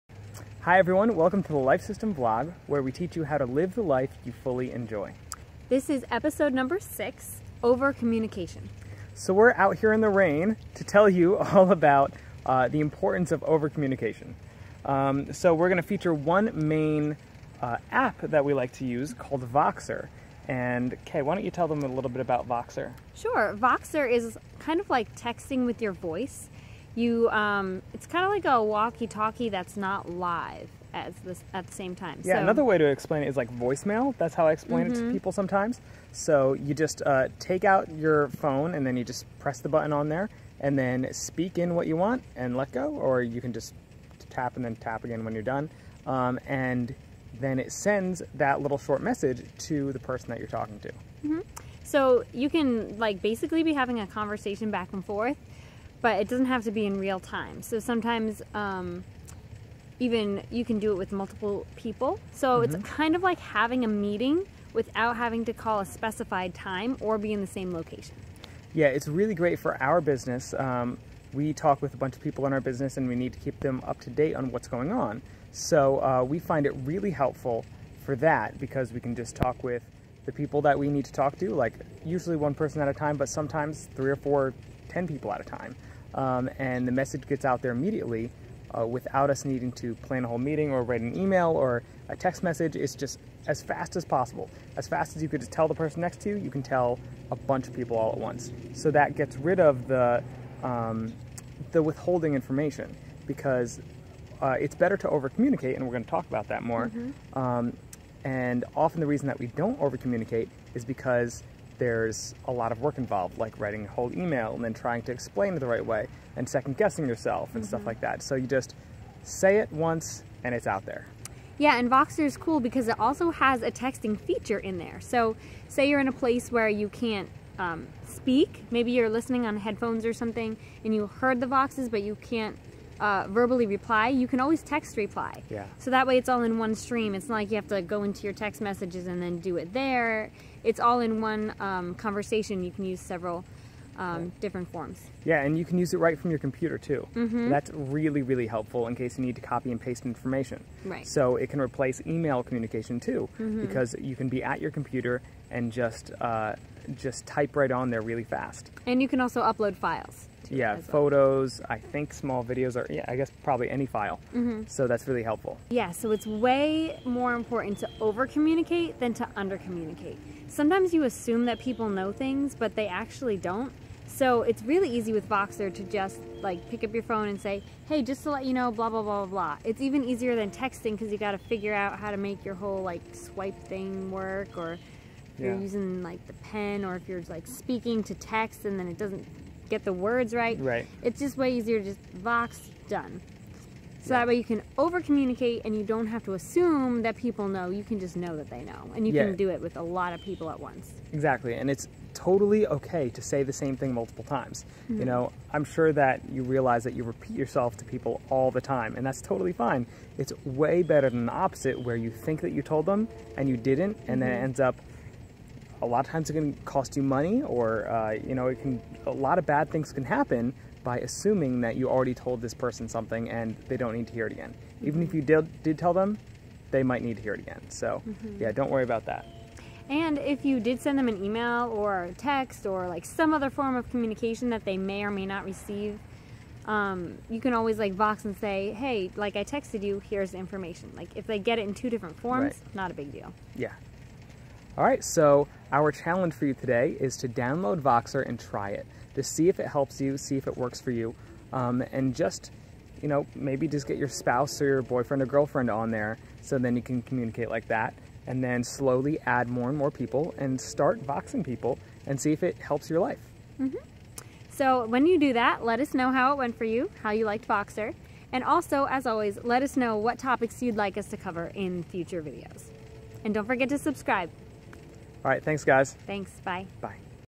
Voxer Overcommunication LYFE System Vlog #6
Overcommunication - Don't assume that people you're talking to remember what you said. It's important to learn the art of overcommunication.

One tool that we recommend for this is Voxer Voxer is amazing! Try it out.